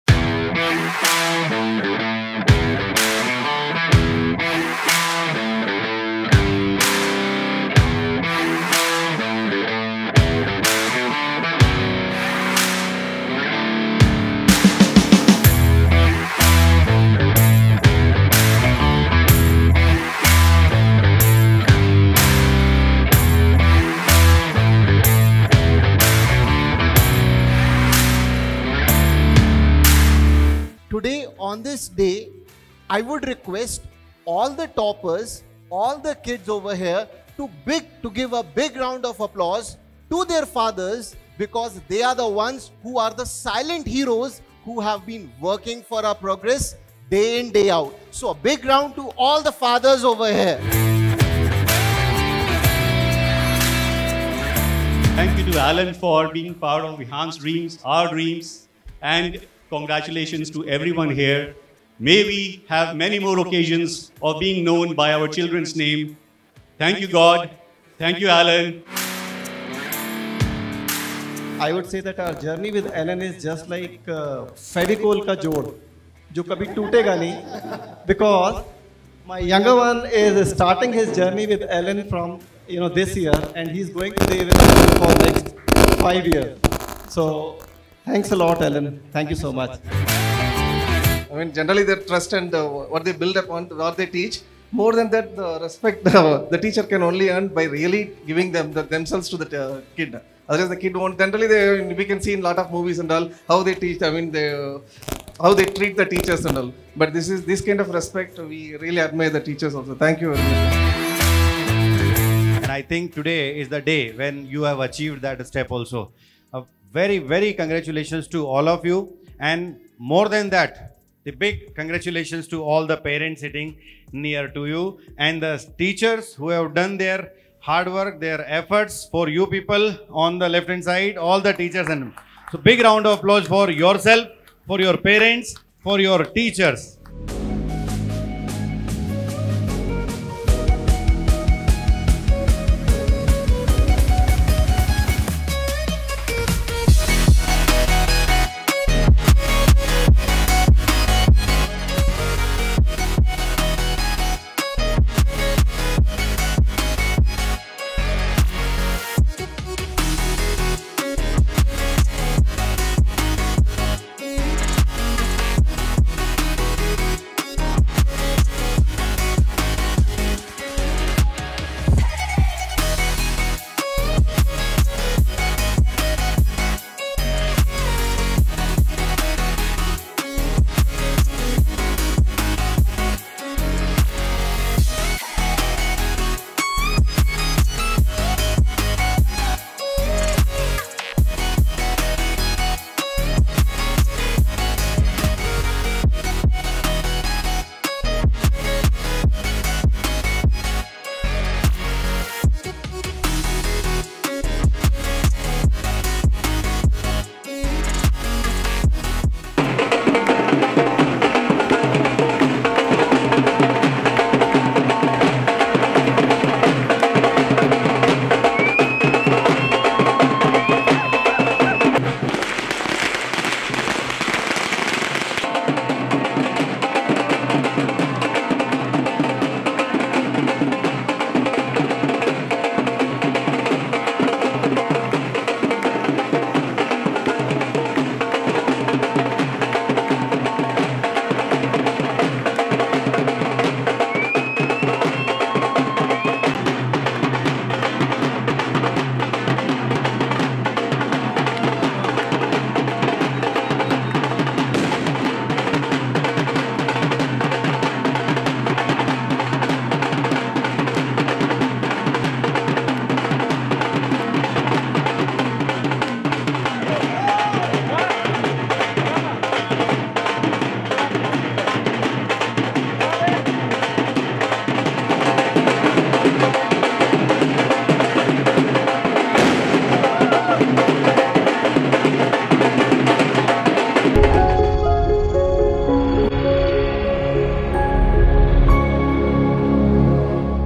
ALLEN Bengaluru JEE (Advanced) 2023 Result Celebration
🙏Once again... ALLEN Bengaluru Mentors have shown great resilience towards their single minded goal of academic excellence.

To Celebrate this tremendous success of JEE Advanced. ALLEN Bengaluru Conducted Mega Result Celebration on 18th June 2023.
☑️ 04 In Top 100 AIR
☑️ 13 In Top 500 AIR
☑️ 30 In Top 1000 AIR
☑️ 116 In Top 5000 AIR
☑️ 202 In Top 10000 AIR
🌟 This Success story is Resultant of Students' Hard work, Grit, Determination and Parents' unstinting support.

🎤Shri. Mahesh Yadav- Head Academics, South India Centers , congratulated Students , Parents and Teachers for stupendous performance.
📷 Here are the glimpses of the ALLEN Bengaluru JEE Advanced 2023 Result Celebration 🎊